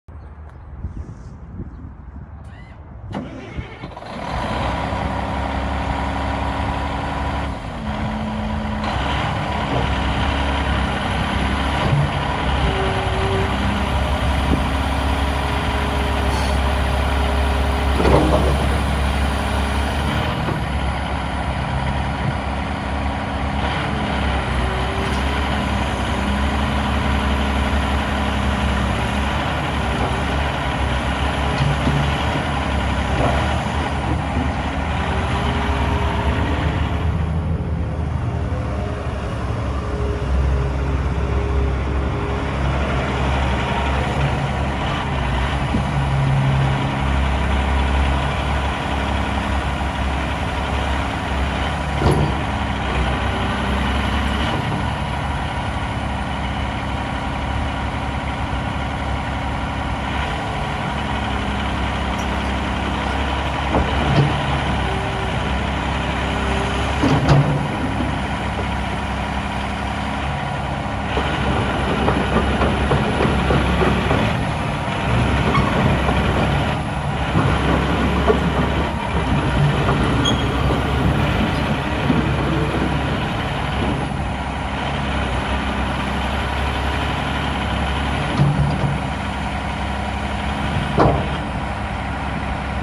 2012 CAT 349EL EXCAVATOR!
THIS IS AN INCREDIBLE 2012 CAT 349EL EXCAVATOR! 115,100LBS! 56" BUCKET! 12'10" LONG STICK! HYDRAULIC COUPLER! HEAT & A/C! BACK UP CAMERA! EQUIPPED WITH A COUNTERWEIGHT REMOVAL SYSTEM! BUY FOR $2,199/MONTH!